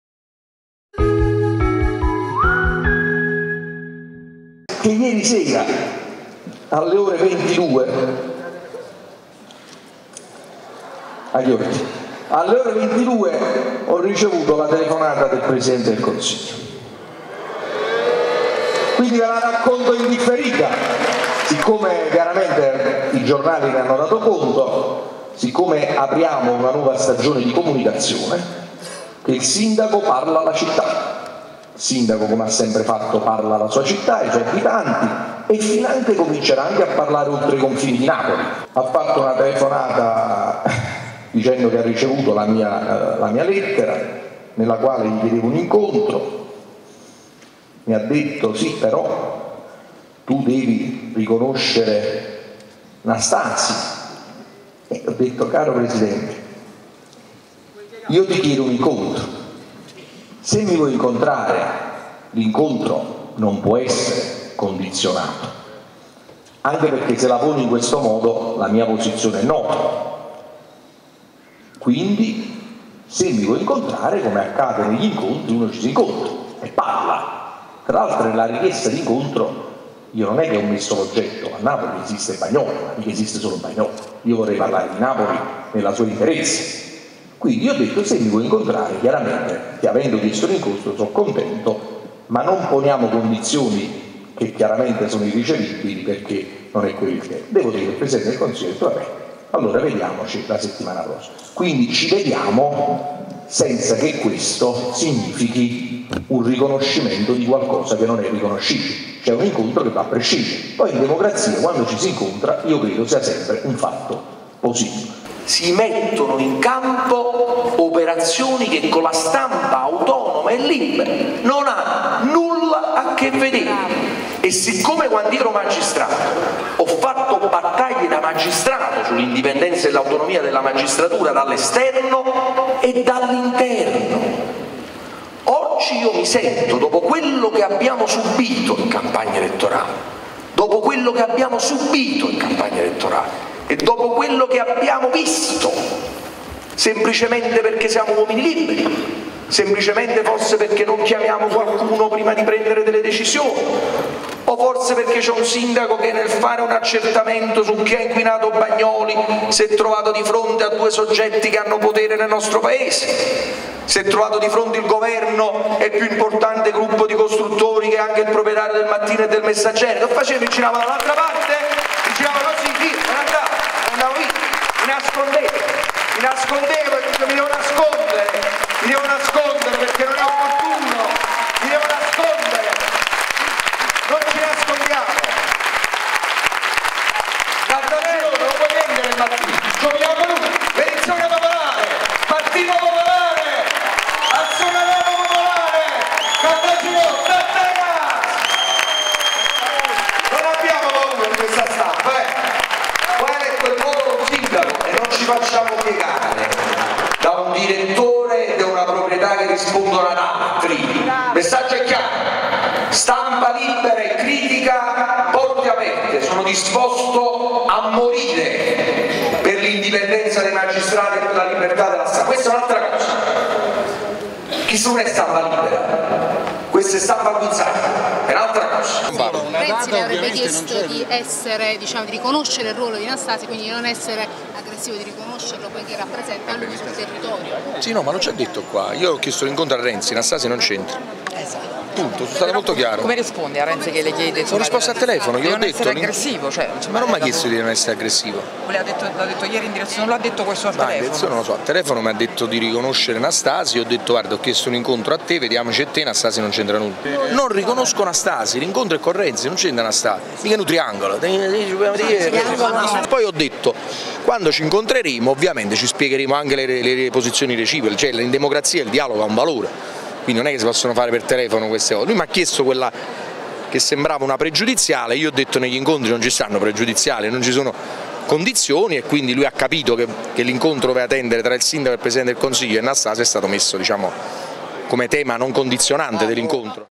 De Magistris parla della telefonata con Renzi, poi attacca Caltagirone: "Stampa al guinzaglio"
"Liberatevi! Le critiche le voglio ma si mettono in campo azioni che con la stampa libera non hanno niente a che fare, non va bene”. Il sindaco grida dal palco dell'Augusteo, durante un'assemblea: “Non abbiamo paura di un direttore e un editore che tengono la stampa al guinzaglio”. E a Caltagirone, proprietario della Cementir destinataria della delibera "Chi inquina paga" su Bagnoli, dice di andare “a casa” per avere il “Mattino libero, Il Mattino popolare”. Su Renzi: "Ieri, alle 22, il presidente del Consiglio mi ha chiamato, dicendomi che aveva ricevuto la lettera che gli avevo inviato, qualche giorno fa, per incontrarlo e parlargli di Napoli. Poi, mi ha detto 'Ti vengo incontro, pero' devi riconoscere Nastasi' (Salvo, commissario straordinario per Bagnoli, ndr.). Poiche' nella lettera non ho parlato solo di Bagnoli, gli ho risposto 'Se mi vuoi incontrare sono contento ma senza pormi condizioni'". Questo il racconto, "in differita", come lo stesso sindaco di Napoli Luigi de Magistris sottolinea, fa alla prima assemblea pubblica dopo la sua rielezione della telefonata di Matteo Renzi. Quindi, "credo che la vittoria popolare ottenuta a Napoli ha avuto il riconoscimento dal Governo". Il primo cittadino chiarisce poi ai cronisti che la sua posizione e' chiara: "L'incontro sara' tra Luigi de Magistris, sindaco di Napoli, e Matteo Renzi, presidente del Consiglio". "Poi ho detto - aggiunge - quando ci incontreremo, ci spiegheremo anche le posizioni reciproche. In democrazia, il dialogo ha un valore. Non e' che si possono fare per telefono queste cose". L'ex pm dice che Renzi "mi ha chiesto quello che a me sembrava una pregiudiziale. Ho risposto che nell'incontro non ci sono pregiudiziali e, quindi, e' stato messo come tema non condizionante".